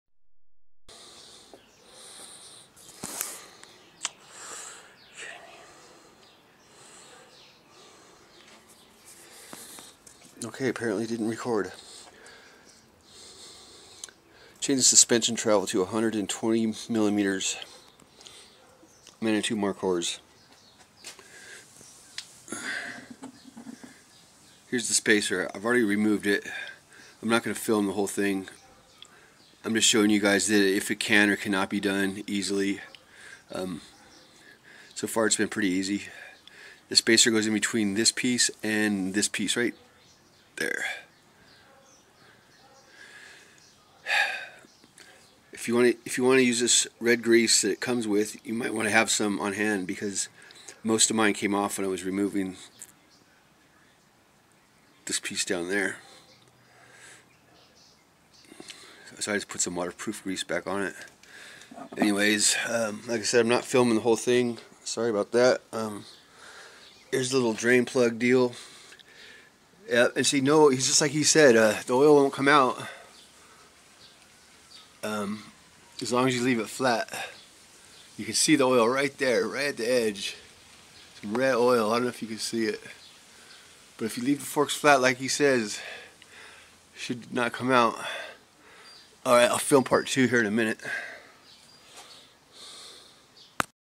Manitou markhor travel change to 120 part 1
these 4 videos are designed to be used after you watch Joe from "take a hike" bike shop video on travel change to 120 mm good luck